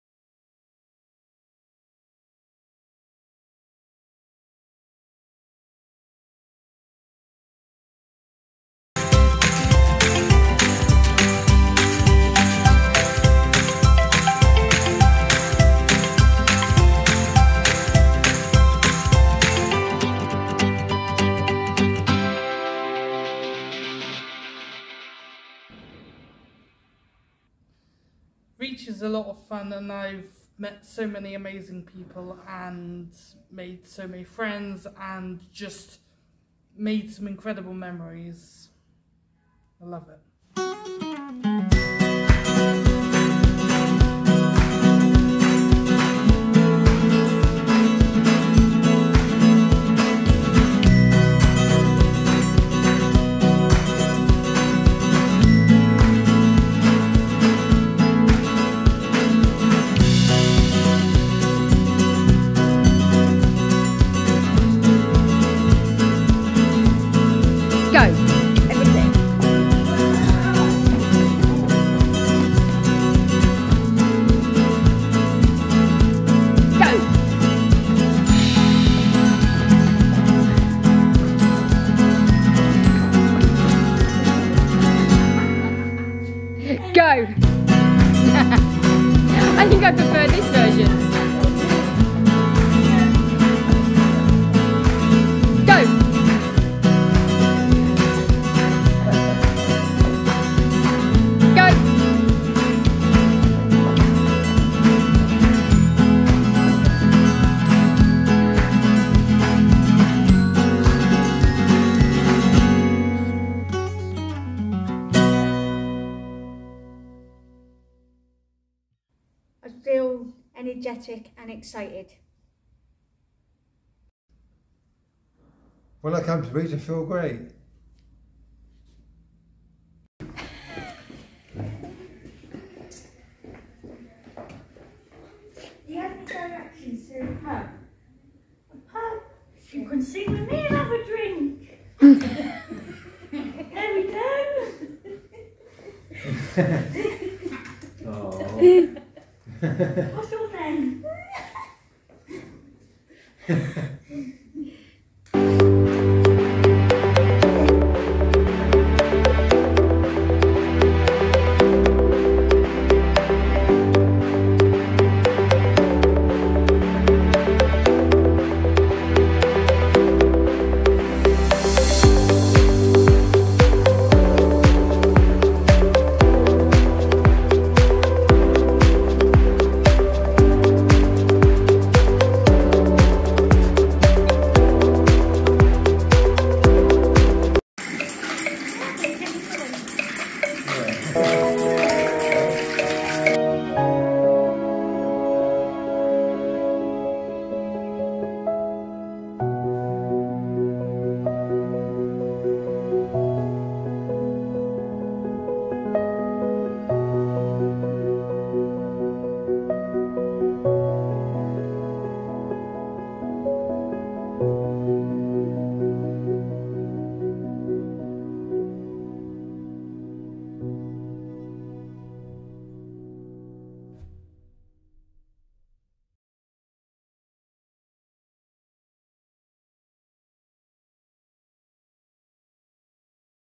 Reach Inclusive Arts Video 2022
Reach Inclusive Arts (RIA) creates opportunities for people, of all ages with a range of disabilities, including learning difficulties, physical disabilities, sensory impairment, people who use mental health services, those who are isolated, and others disadvantaged in society, to experience a range of artistic mediums.